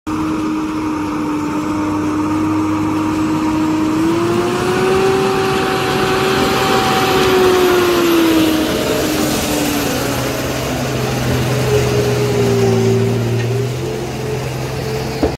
Truck In Mud | Tata Motors 14 Wheeler Load Truck Live On Mud Road | Indian Truck Video.
Indian Heavy Truck Live Running Video On Mud Road.
In This Truck In Mud Video, 14 Wheeler Load Truck Slipt deadly.